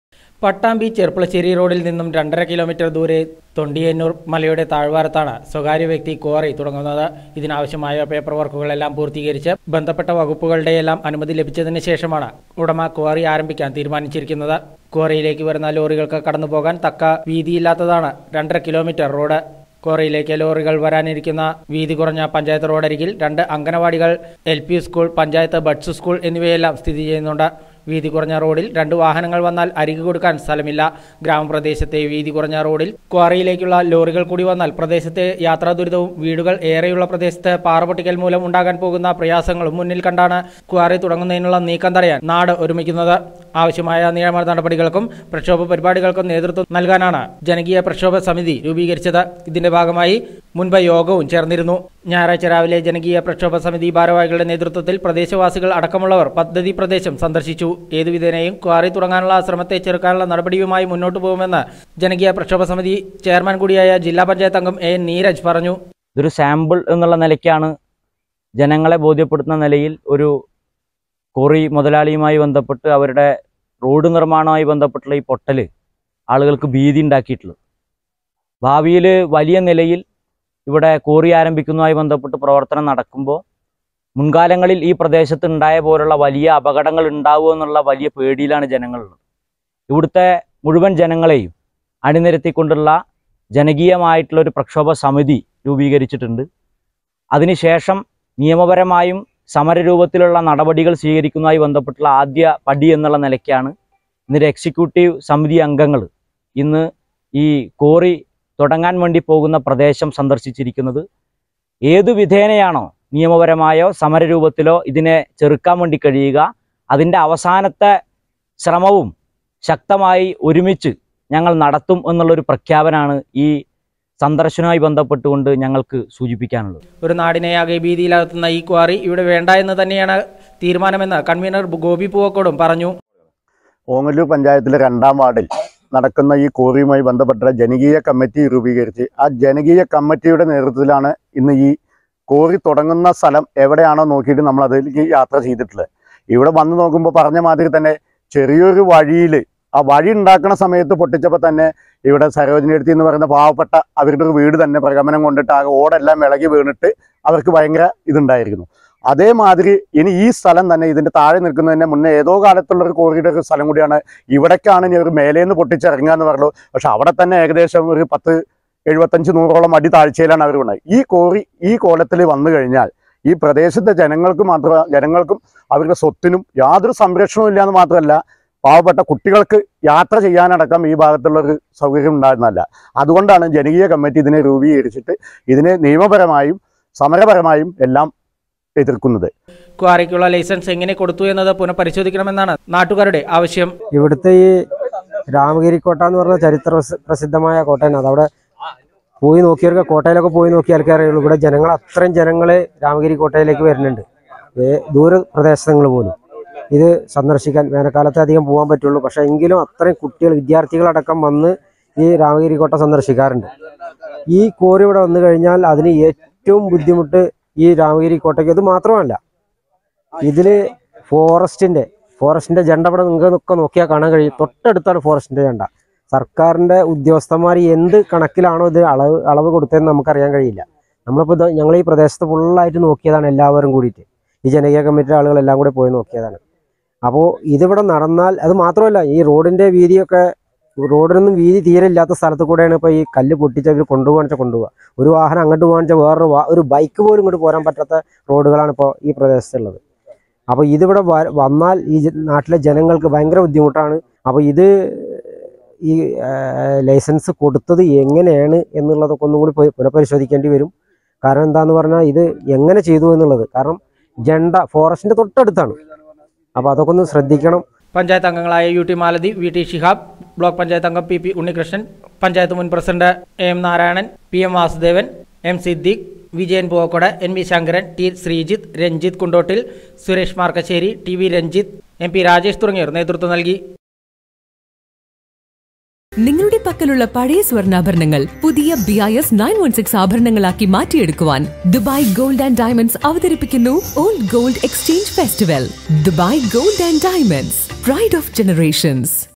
കരിങ്കൽ ക്വാറി ആരംഭിക്കാനുള്ള നീക്കത്തിനെതിരെ പ്രതിഷേധം ശക്തമാക്കാൻ ജനകീയ പ്രക്ഷോഭ സമിതി
⁣മരുതൂർ തൊണ്ടിയന്നൂരിൽ കരിങ്കൽ ക്വാറി ആരംഭിക്കാനുള്ള നീക്കത്തിനെതിരെ പ്രതിഷേധം ശക്തമാക്കാൻ ജനകീയ പ്രക്ഷോഭ സമിതി